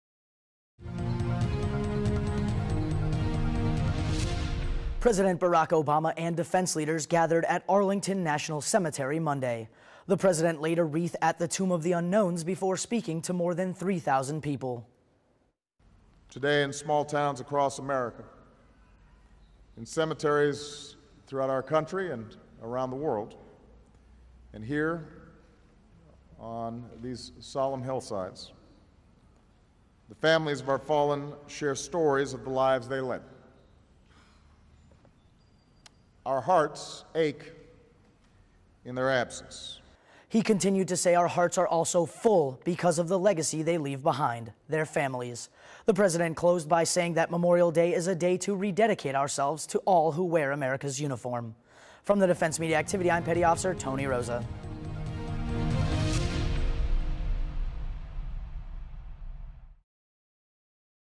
President Obama's Memorial Day Address: " Our Heart Aches in their Absence"
Headlines for Tuesday May 27, 2014: President Obama's Memorial Day Address: " Our Heart Aches in their Absence"